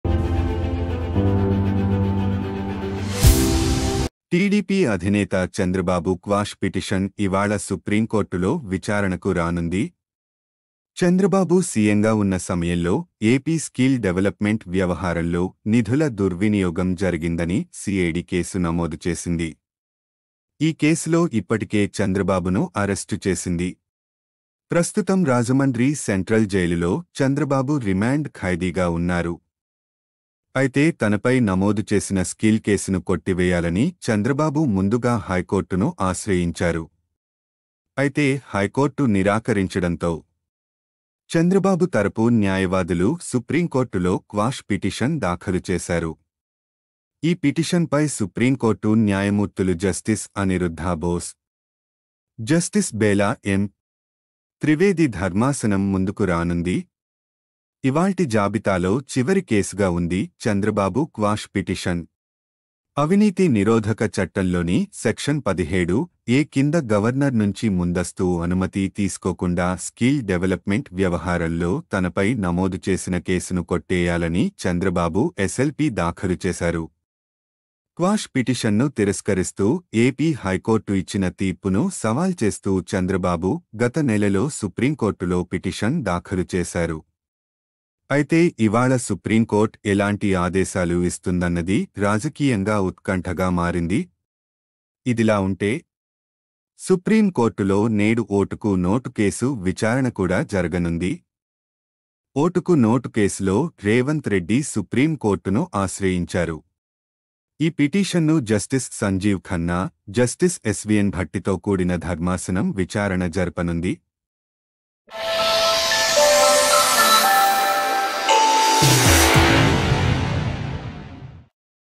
బాబుకు దెబ్బ మీద దెబ్బ | ఈ రోజు చివరి అవకాశం | NPN Live News
NPN Live News
ఎన్ పి ఎన్ లైవ్ న్యూస్

టీడీపీ అధినేత చంద్రబాబు క్వాష్ పిటిషన్ ఇవాళ సుప్రీంకోర్టులో విచారణకు రానుంది. చంద్రబాబు సీఎంగా ఉన్న సమయంలో ఏపీ స్కిల్ డెవలప్‌మెంట్‌ వ్యవహారంలో నిధుల దుర్వినియోగం జరిగిందని సీఐడీ కేసు నమోదు చేసింది. ఈ కేసులో ఇప్పటికే చంద్రబాబును అరెస్టు చేసింది. ప్రస్తుతం రాజమండ్రి సెంట్రల్ జైలులో చంద్రబాబు రిమాండ్ ఖైదీగా ఉన్నారు

Revanth Reddy approached the Supreme Court in the case of notes for votes.